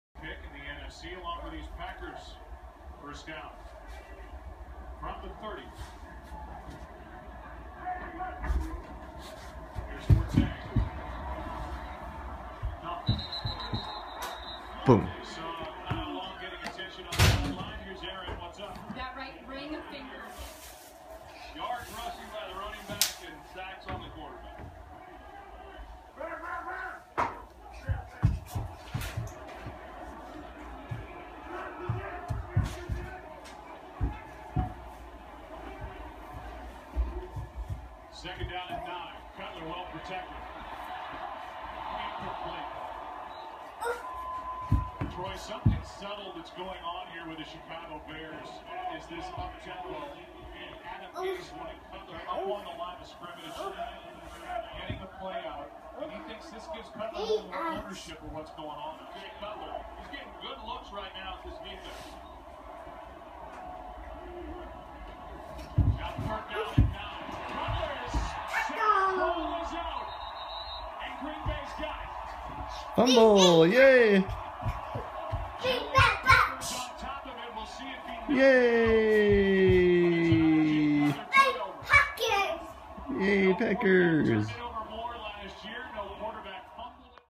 Jaxton interprets Packers-Bears game as it happens
Toddler interprets Packers-Bears game as it happens.